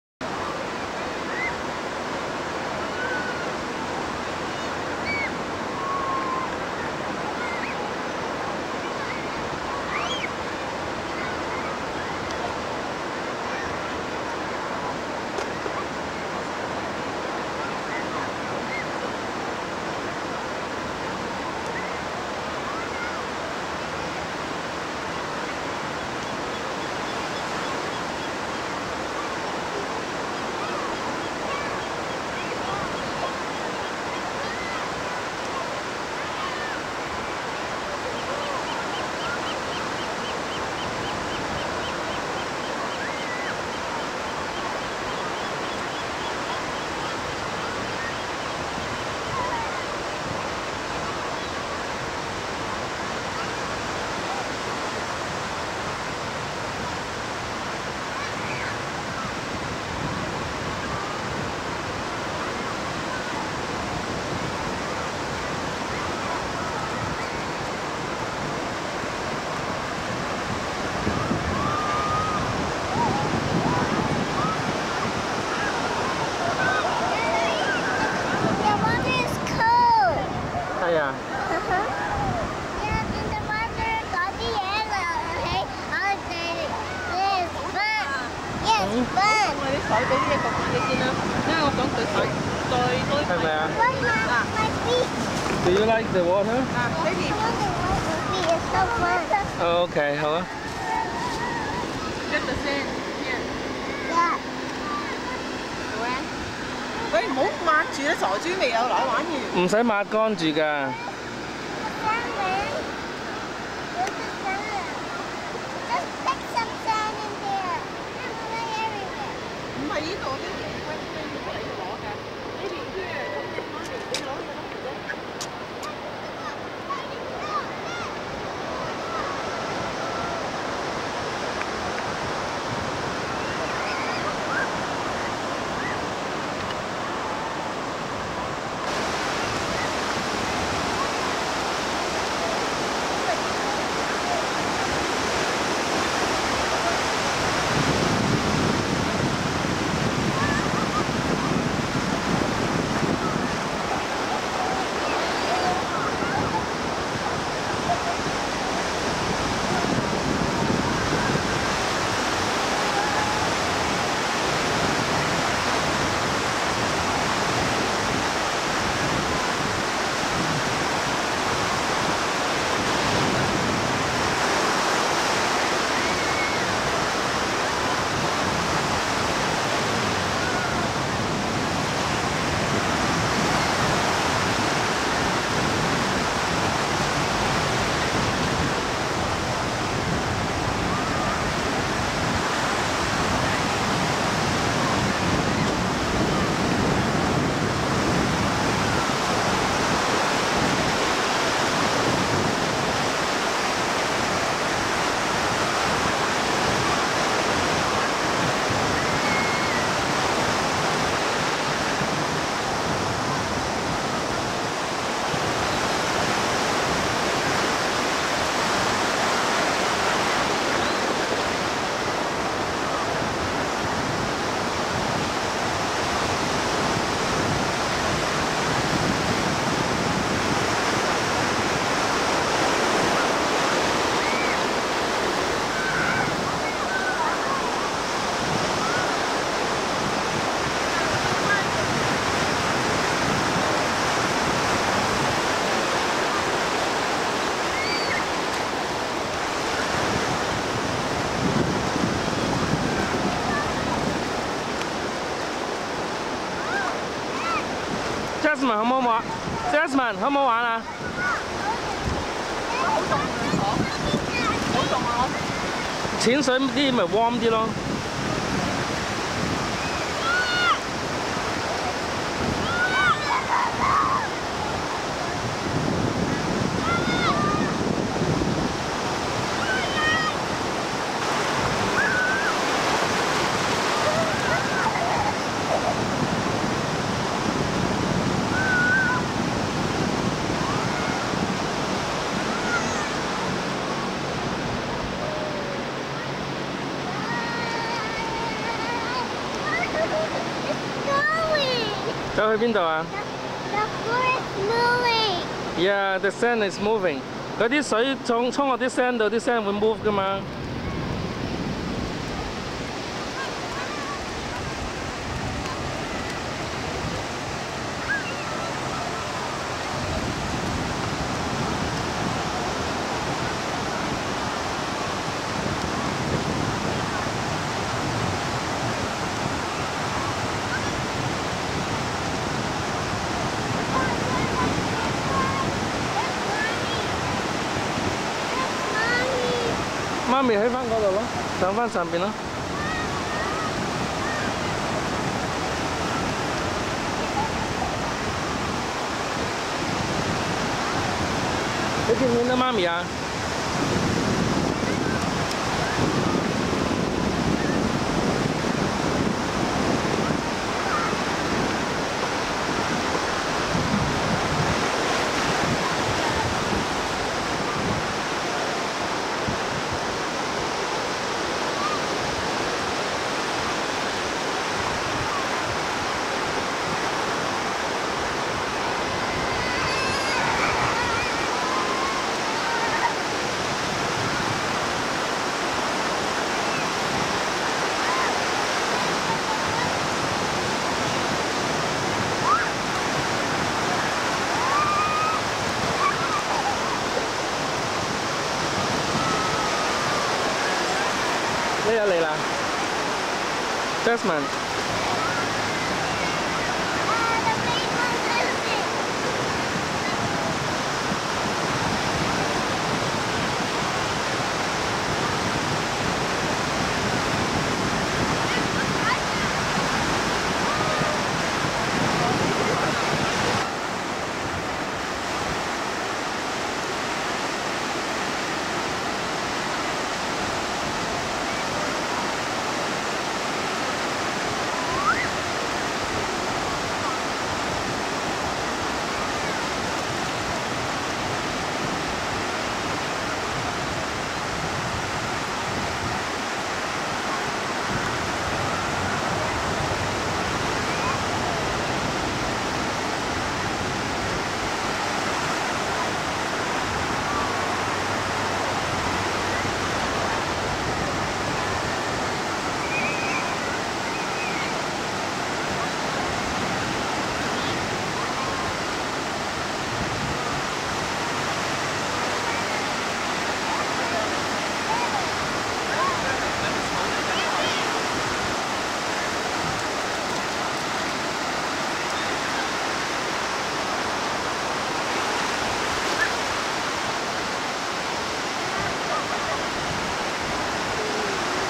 4 yo twins play on La Jolla Shore Beach
4 year old twins played in the ocean for the first time.
I believe it is La Jolla Shore Beach in San Diego.

Hi8 tape 23 clip 4 2002/06/25